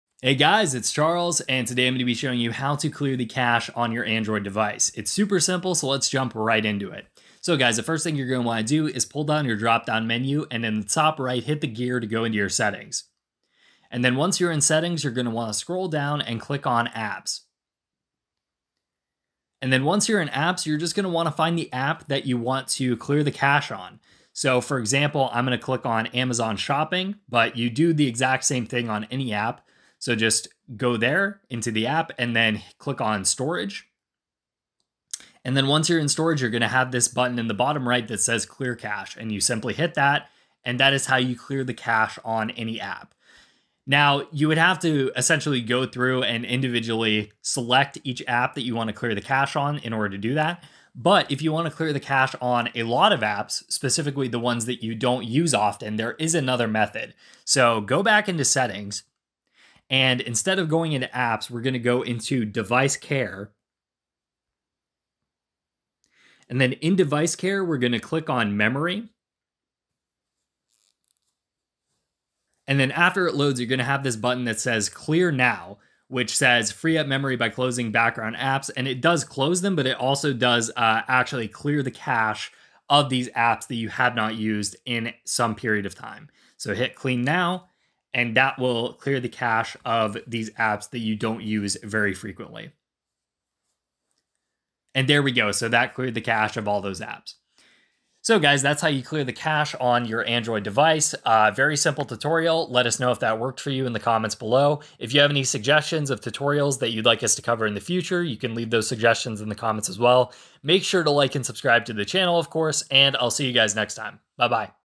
How To Clear Cache On Android
Let's clear your cache on any Android device in this quick and easy guide.

Whether you're on an Android phone or a tablet, I walk you through steps to clear the cache. You can clear your data or cache for a specific app like Play Store or YouTube or you can clear it for all the apps and clear up some memory. This process works on any Android from Samsung to Huawei in 2021.